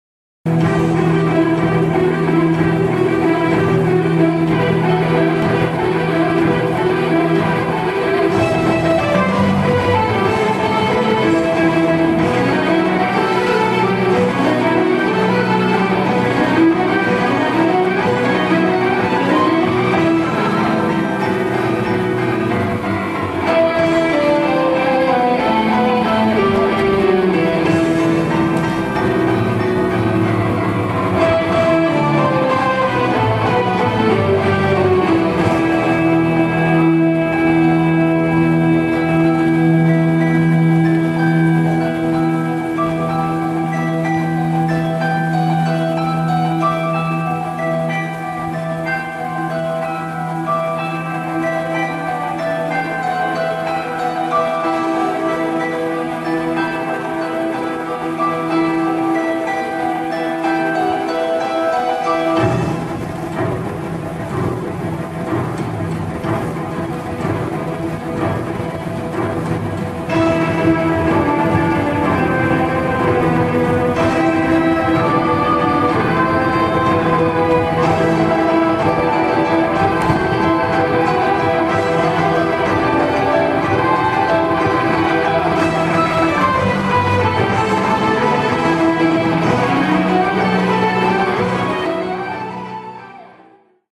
The Little Matchgirl.wmv
The Little Matchgirl is a musical adaptation of Hans Christian Anderson's famous story  "The Little Match Girl." It is a contemporary rock opera set to the music of the Trans-Siberian Orchestra and is told through narrative and movement.

It is directed and choreographed each year by January Wolfe and Dorothea Hereford with guest choreography by Heather Taylor, Lorien McDavid and Maggie Bryan.